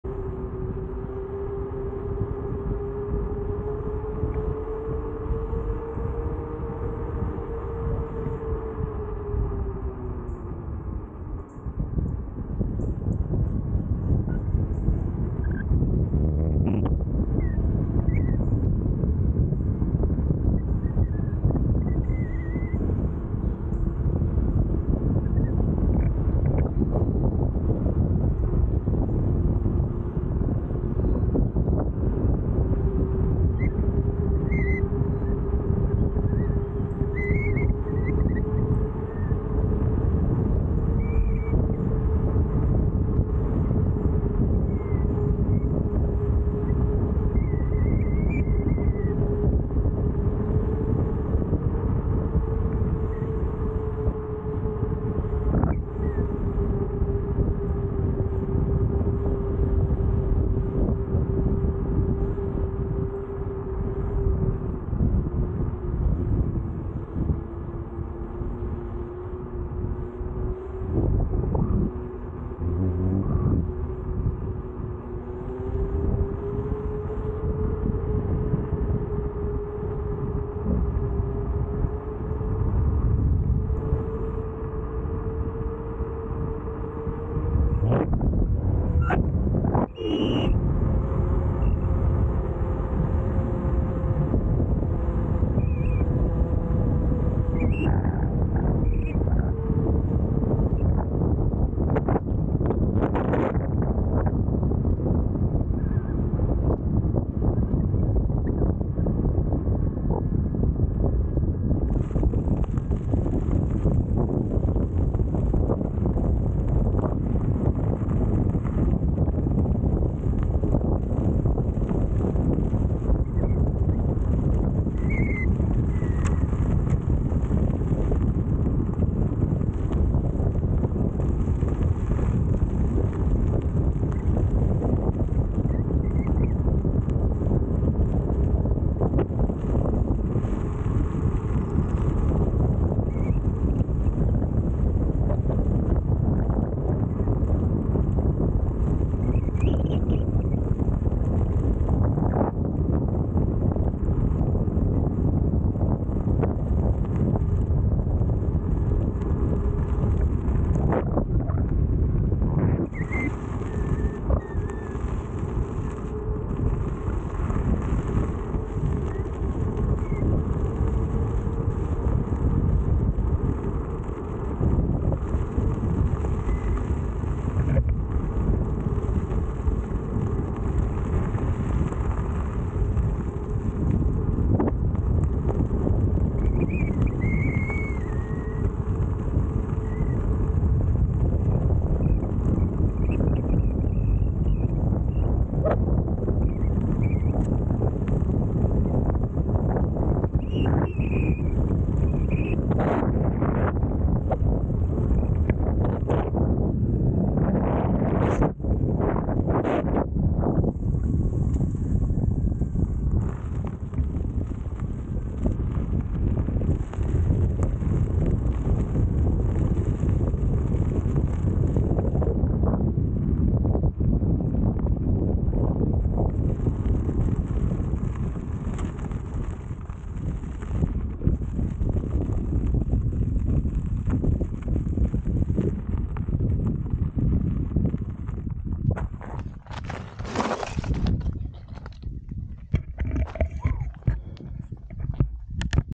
KingSong S22 Wobble practice (pt. 1)
Tried to keep it above 40kmph, got it up to 56kmph comfortably.  I'll have to play with tire pressure and suspension along with foot placement to minimize wobbles at higher speeds.